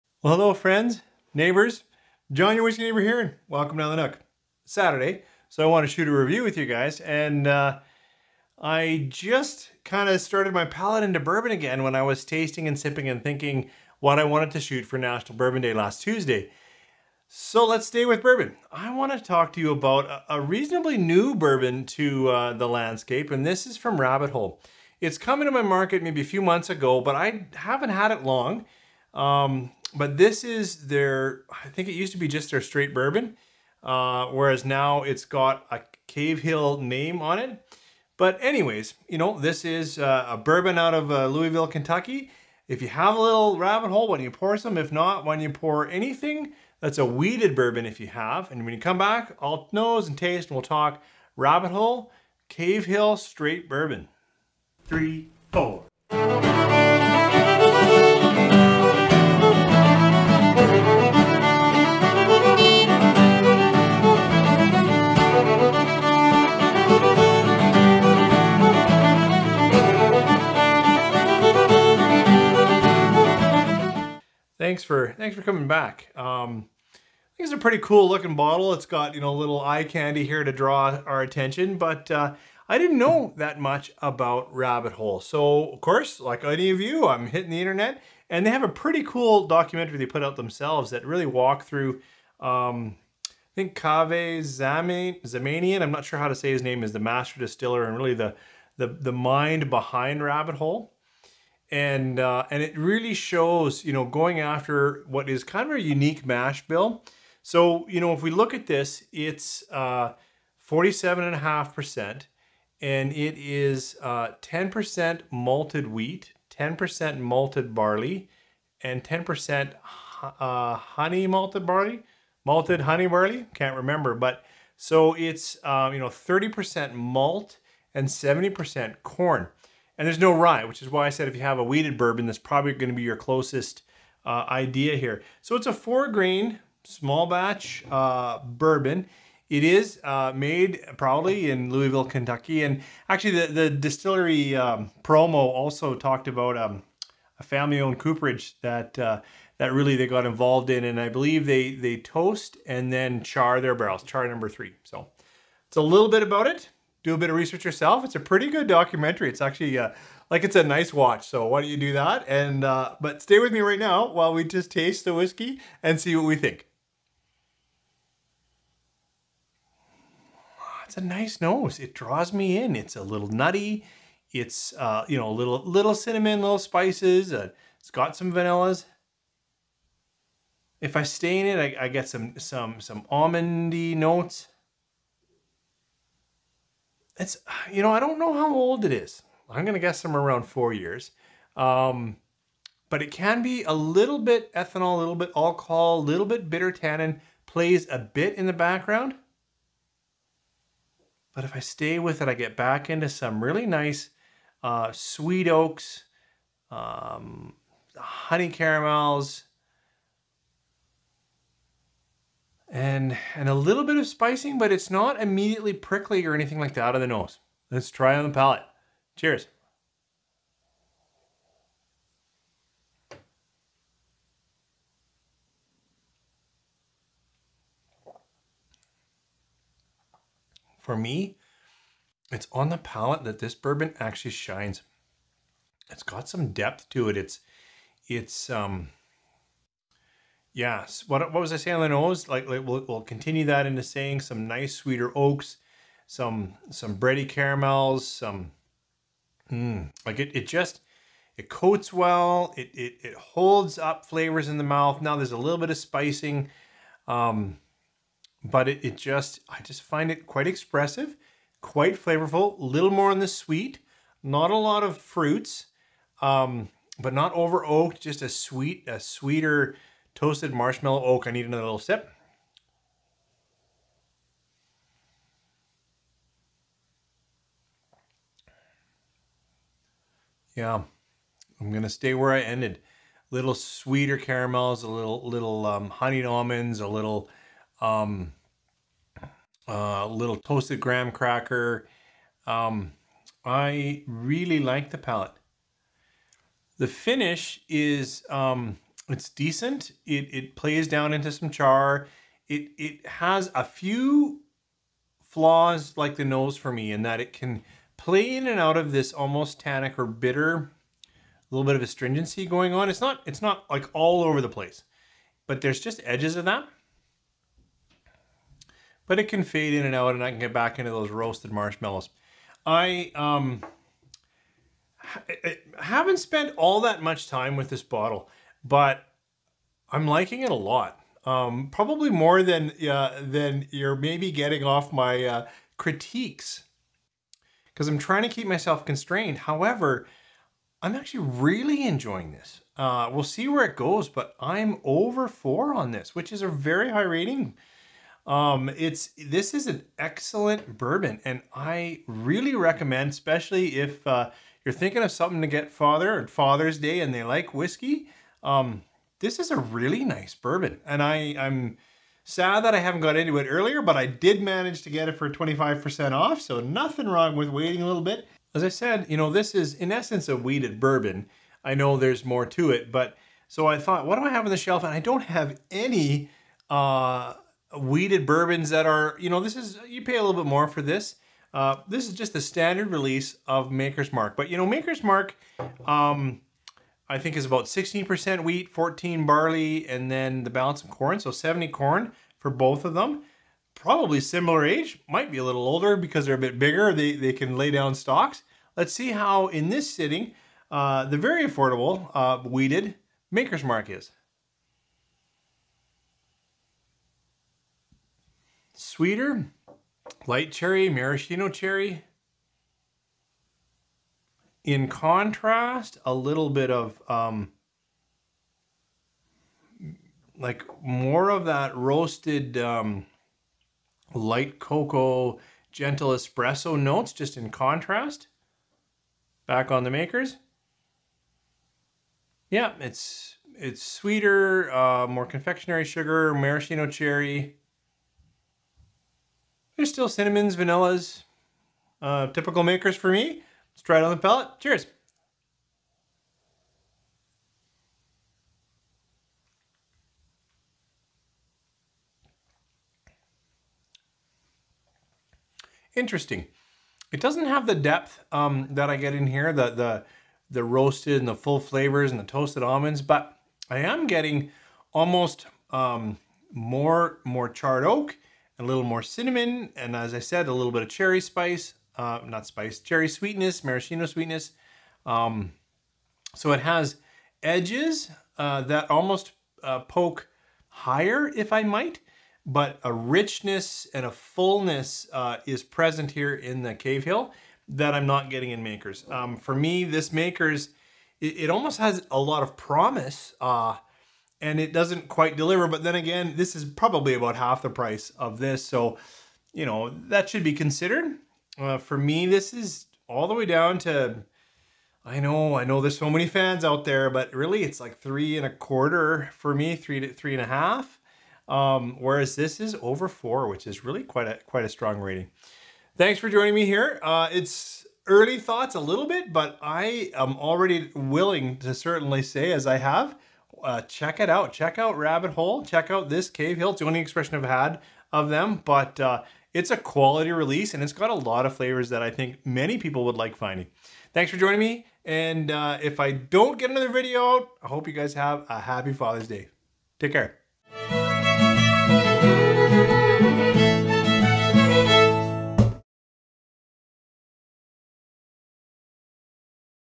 Rabbit Hole Cavehill Straight Bourbon
Episode 405. I have not spent a lot of time with this release , but I am really enjoying my first drams. More depth than most, it plays into a nicely roasted, wheated bourbon profile. Have you had this release? What are your thoughts on Rabbit Hole or Cavehill?